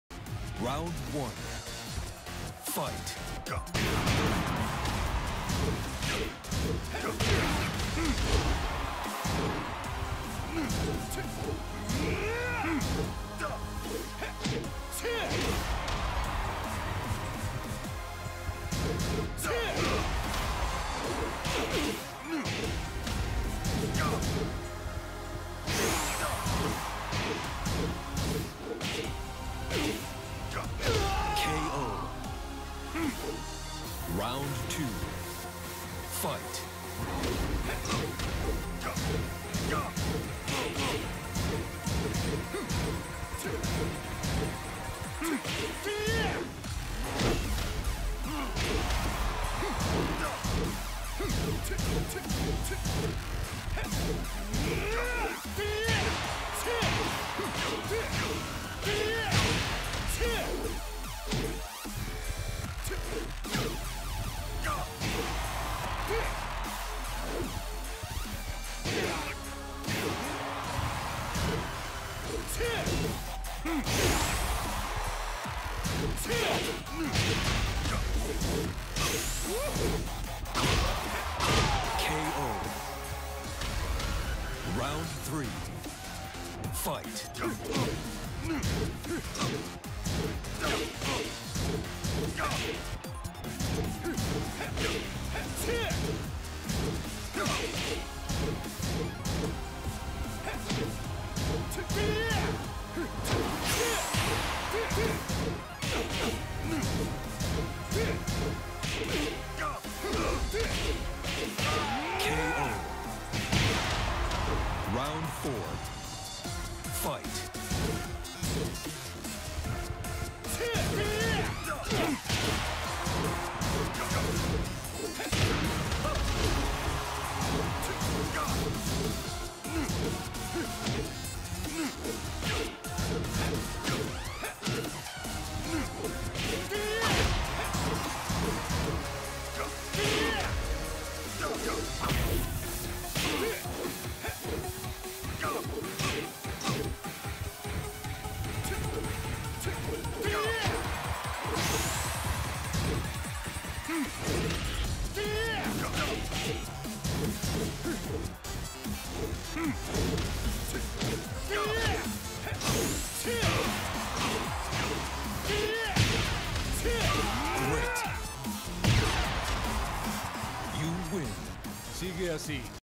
abelity (Miguel) vs Jacket (Marduk) - Offline Casual
Had a 6 hours OFFLINE session yesterday to "prepare" myself for ONLINE tournament lol

I might've won this match, but this was the very last match of the day where both of us were mentally tired already. Overall we played 92 matches, with me losing 6-86.

In the future I might wanna do more FT10s (or double FT5s) against various characters and then review the replays accordingly (preferably with the person I played with).